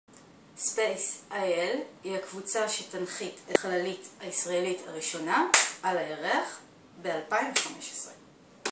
ספייס איי אל בשפת הסימנים
ספייס איי אל היא העמותה שתנחית את החללית הישראלית הראשונה על הירח ב-2015!